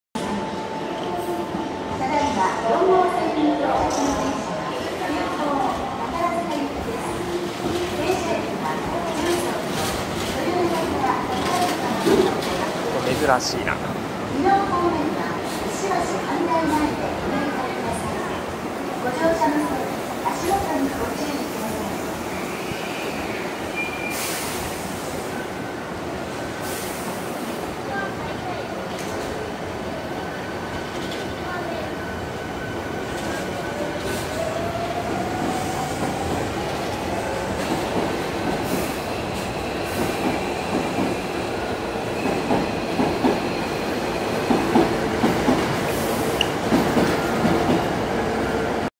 祇園祭ＨＭ付阪急宝塚本線８０００系未更新ラスト１編成８００５Ｆ[急行宝塚行き]阪急大阪梅田駅到着
２０２４年７月１３日(土曜日)